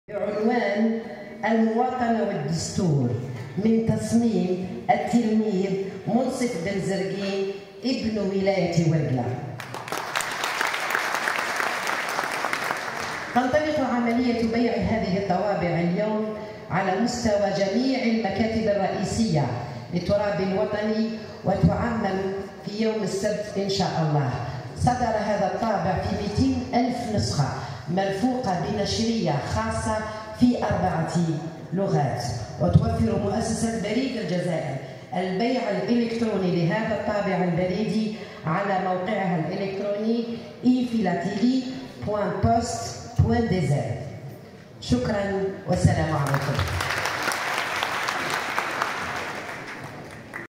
بريد الجزائر يصدر طابع بريدي بعنوان المواطنة والدستورمن تصميم تلميد بالمرحلة المتوسطة
بريد الجزائر يصدر طابع بريدي بعنوان المواطنة والدستورمن تصميم تلميد بالمرحلة المتوسطة من ولاية ورقلة جنوب شرقي الجزائر